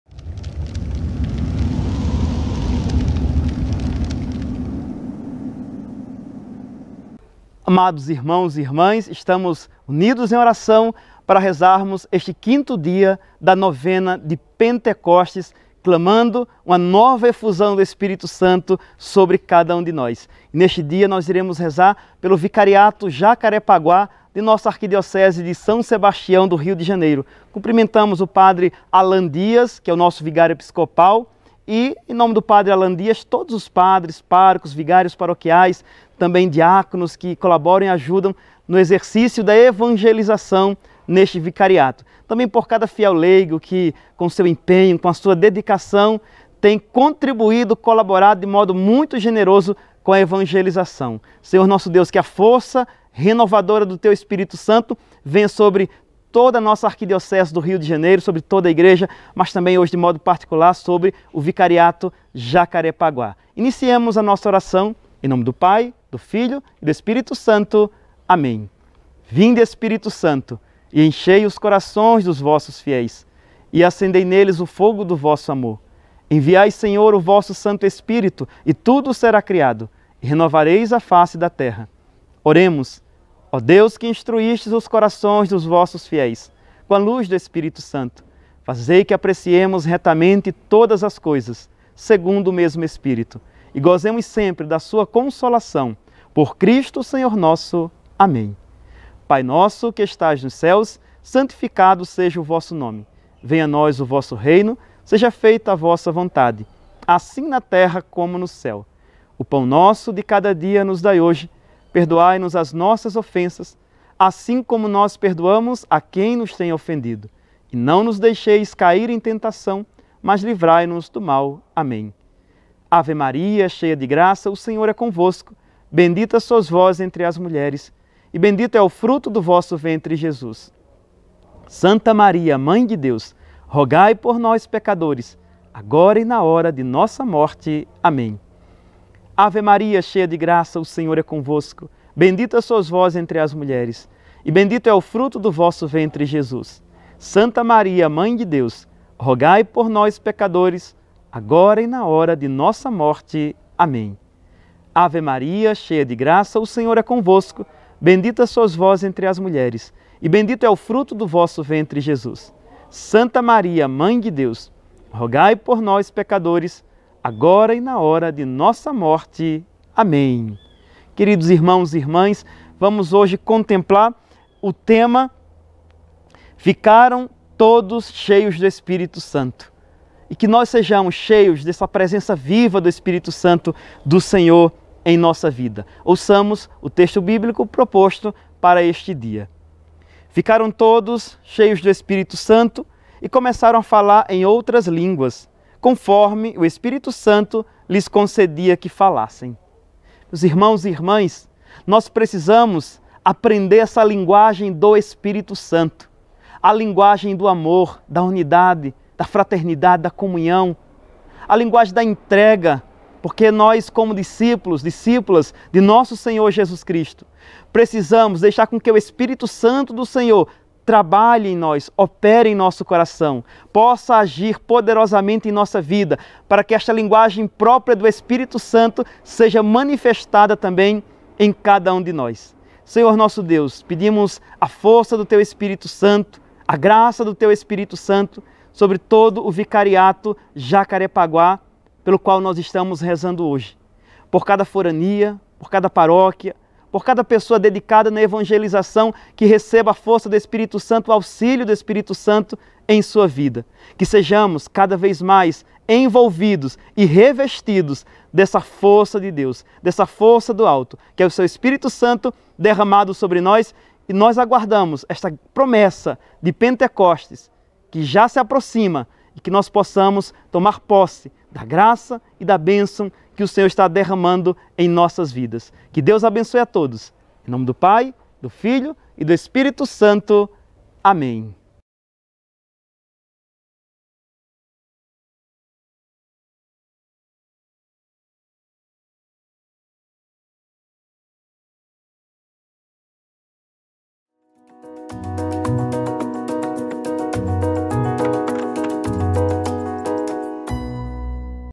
5º Dia da Novena de Pentecostes 2022 na WebTV Redentor | ArqRio | 31/05/2022
Dia 5 – Tema: Ficaram todos cheios do Espírito Santo (AT 2,4)
Vicariato Jacarepaguá

Em parceria com a TV Século 21, a WebTv Redentor preparou uma caminhada de oração, para a grande celebração de Pentecostes. Com apresentação do Padre Jadilson Ferreira, acompanhe de 27 de maio até 4 de junho, sempre às 9h da manhã.

WebTV Redentor:
Youtube.com/WebTVRedentor
Ative as notificações clicando no sino!